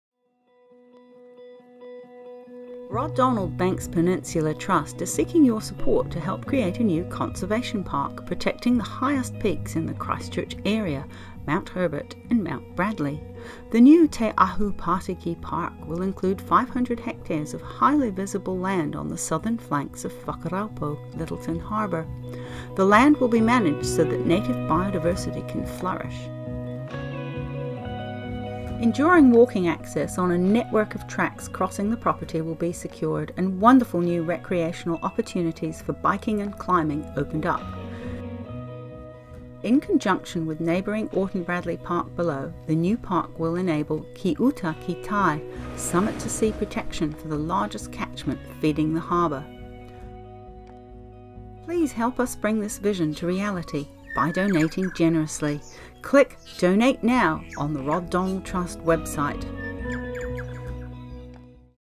Te Ahu Pātiki | Mt. Herbert - Protecting the Highest Peaks of Banks Peninsula
Te Ahu Pātiki is 500ha of iconic land including the summits of Mt Herbert/ Te Ahu Pātiki and Mt Bradley, the two highest points in the Christchurch/Ōtautahi district.

Rod Donald Banks Peninsula Trust is proud to announce an agreement with the landowner to purchase this substantial block and bring it into public ownership.

The Trust has committed to fund half the purchase price, neighbouring Orton Bradley Park have pledged a substantial contribution and we are  fundraising the balance of $600,000 to complete the purchase and set up the park. A huge thank you to everyone who has donated so far – we have raised over half of our target now.

We seek your support to realise our vision for this land to become a conservation park protecting and restoring native biodiversity, with full permanent public access.